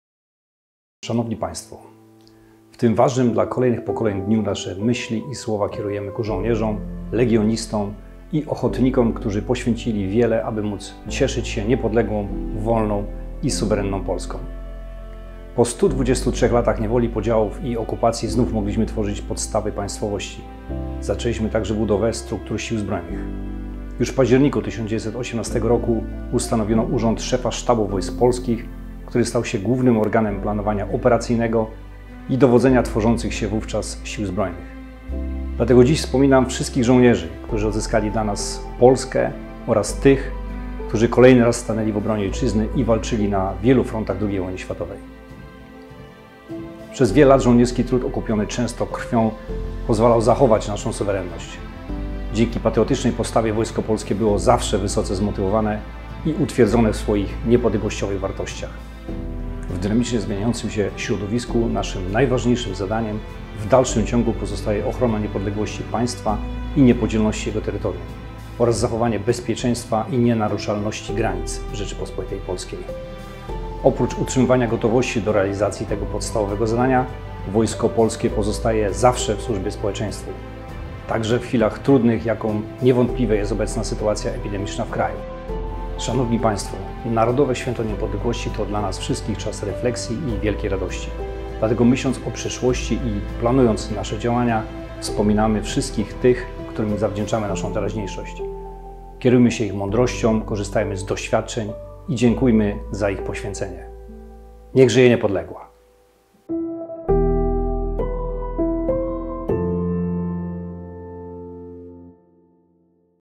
Narodowe Święto Niepodległości - 11 listopada 2020 r
W tym ważnym dla kolejnych pokoleń dniu nasze myśli i słowa kierujemy ku żołnierzom, legionistom i ochotnikom, którzy poświęcili wiele aby móc cieszyć się niepodległą, wolną i suwerenną Polską.

Po 123 latach niewoli, podziałów i okupacji znowu mogliśmy tworzyć podstawy państwowości. Zaczęliśmy także budowę struktur sił zbrojnych.

Już w październiku 1918 roku ustanowiono urząd Szefa Sztabu Wojsk Polskich, który stał się głównym organem planowania operacyjnego i dowodzenia tworzących się wówczas sił zbrojnych.

Dlatego dziś wspominam wszystkich żołnierzy, którzy odzyskali dla nas Polskę oraz tych, którzy kolejny raz stanęli w obronie Ojczyzny i walczyli na wielu frontach II wojny światowej.

Przez wiele lat żołnierski trud, okupiony często krwią, pozwalał zachować naszą suwerenność.

Dzięki patriotycznej postawie Wojsko Polskie było zawsze wysoce zmotywowane i utwierdzone w swoich niepodległościowych wartościach.

W dynamicznie zmieniającym się środowisku, naszym najważniejszym zadaniem w dalszym ciągu pozostaje ochrona niepodległości państwa i niepodzielności jego terytorium oraz zapewnienie bezpieczeństwa i nienaruszalności granic Rzeczypospolitej Polskiej.
Oprócz utrzymywania gotowości do realizacji tego podstawowego zadania, Wojsko Polskie pozostaje ZAWSZE w służbie społeczeństwu.

Także w chwilach trudnych, jaką niewątpliwie jest obecna sytuacja epidemiczna w kraju.

Narodowe Święto Niepodległości to dla nas wszystkich czas refleksji i wielkiej radości.

Dlatego myśląc o przyszłości i planując nasze działania, wspominamy wszystkich tych którym zawdzięczamy naszą teraźniejszość.

Kierujmy się ich mądrością, korzystajmy z doświadczeń i dziękujmy za ich poświęcenie.

Niech żyje Niepodległa!

Szef Sztabu Generalnego Wojska Polskiego
generał Rajmund T. Andrzejczak